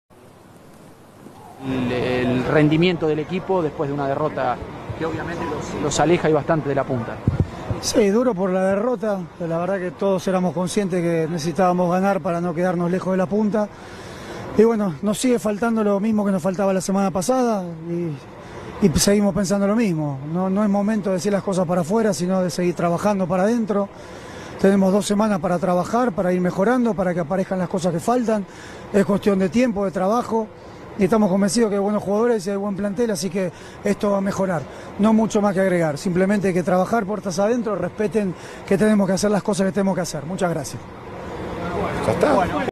Cocca DT de Racing , entrevista ∆Argentinos VS Racing∆ Fox Sports
Fox Sports